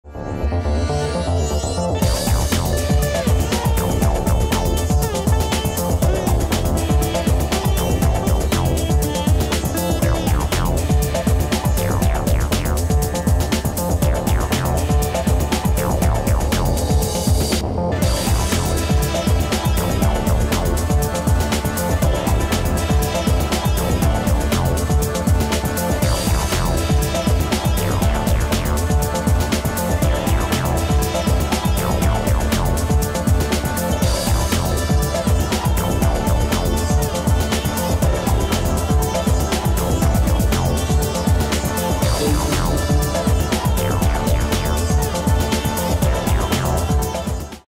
Sexiest Way EVER To Watch TV - Forget About The Pricetag
Shadow Dancers Wow Factor provides visuals in a 9:16 aspect ratio that will make your jaw drop, they are so evocative that punters stop in their tracks. this 2 DVD Auto looping set is over 2 hrs of the hottest dancers.